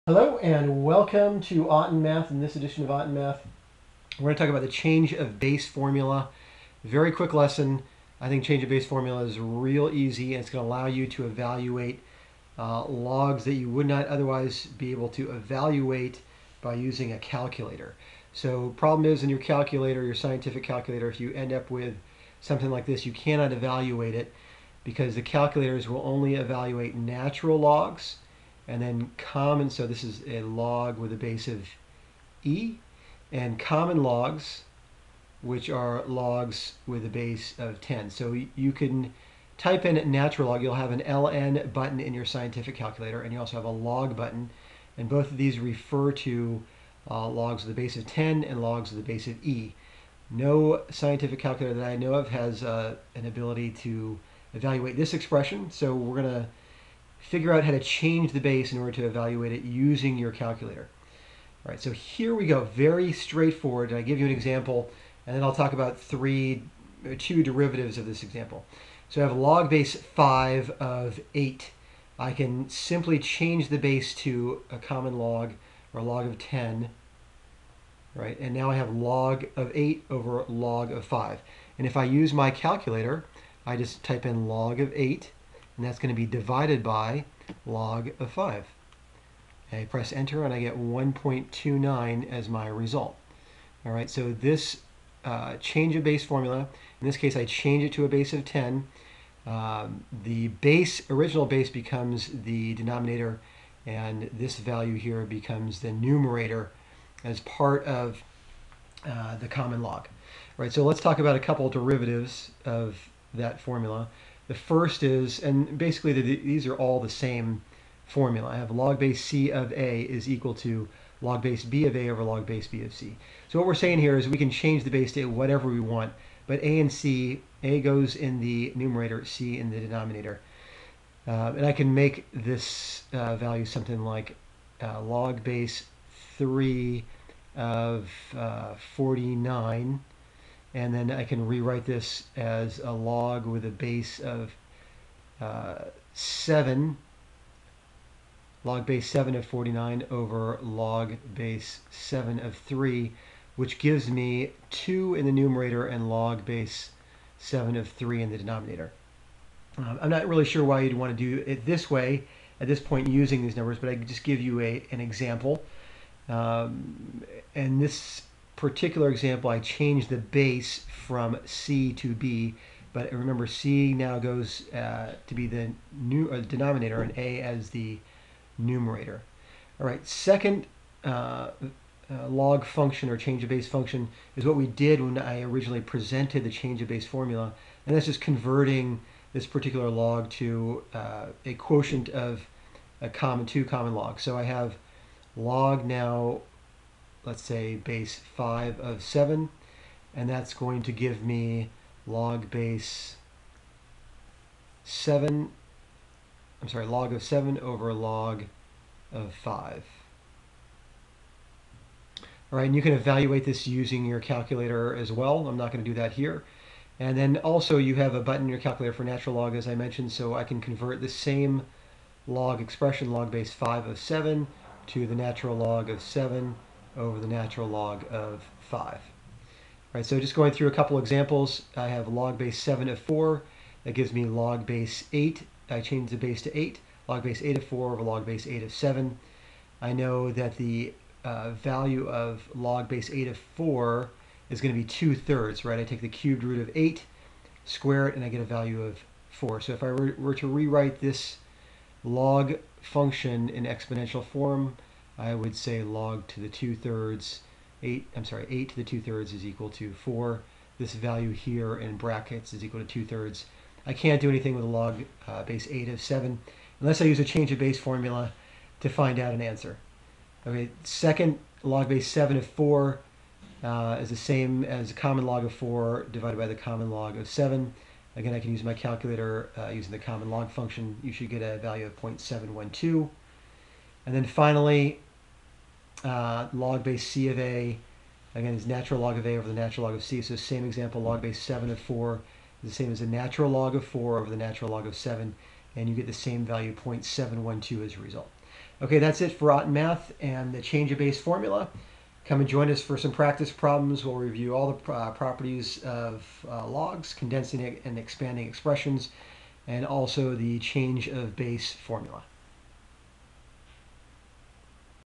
7.5 Apply Properties of Logs: The Change of Base Formula (Lesson)
A lesson on applying properties of logs using the change of base formula